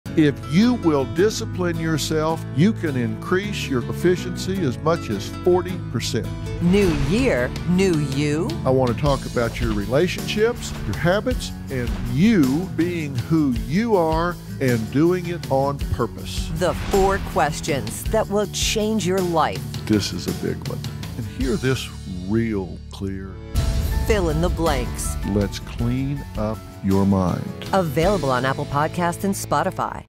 Efficiency For 2022
Listen in to this important episode that will help you make 2022 the year you need it to be.  Dr. Phil provides insights into how to make your life more efficient by letting go of multi-tasking, mental clutter, toxic people, and resolving lingering personal issues that distract and muddy the waters.  He’ll help you stay focused, on purpose, and inspired to dare greatly and own the unique person you are that can bring in all the success you hope for!

Thank you for your support!

Interested in advertising on the show?